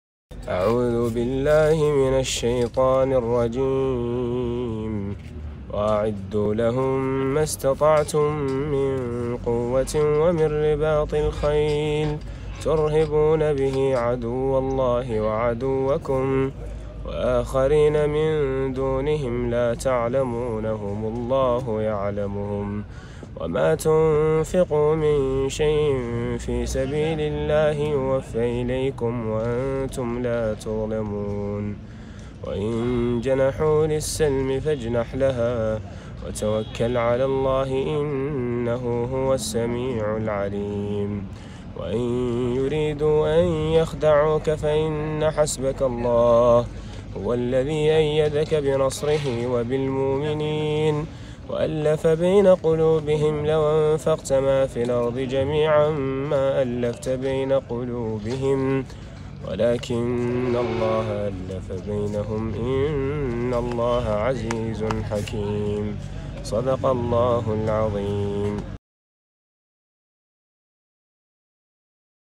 تلاوة التلميذ الدركي
تلاوة التلميذ الدركي، سيد أحمد ولد أواه، خلال حفل تخرج الدفعة الـ51 من تلاميذ مدرسة الدرك الوطني بروصو.